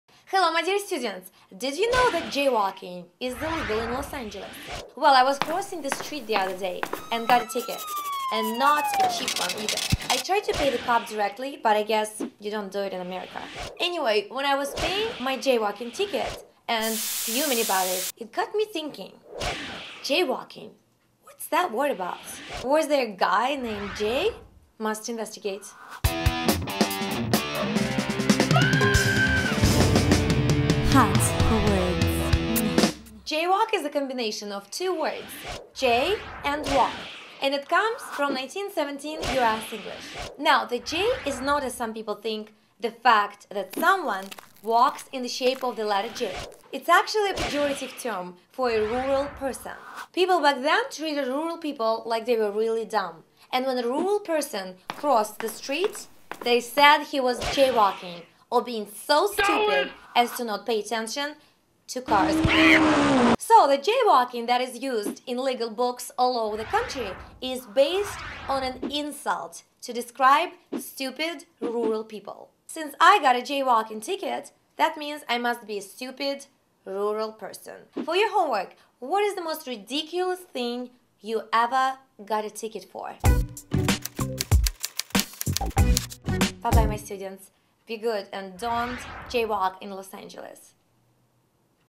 Origin of jaywalking
Did you know that jaywalking is illegal in Los Angeles? Well I was crossing the street the other day and got a ticket. And not a cheap one either. I tried to pay the cop directly but I guess you don'd do it in America. Anyway when I was paying my jaywalking ticket and fuming about it, it got me thinking...Jaywalking. what is that word about? Was it a guy named Jay? Must investigate!
Jaywalk is a combination of two words: jay and walk. And it comes from 1917 US English. Now, J is not what some people think the fact that someone walks in a shape of the letter J. It is actually a pejorative term for a rural person. people back then treated rural people like they were really dumb. When a rural person cross the street, they said he was JAYWALKING or being so stupid as to not pay attention to cars.
So the jaywalking that is used in legal books all over the country is based on an insult to describe stupid, rural people. If I got a jaywalking ticket, that means I must be a stupid rural person.
For your homework, what is the most ridiculous thing you ever got a ticket for?
Bye, bye my student, be good and don't jaywalk in Los Angeles!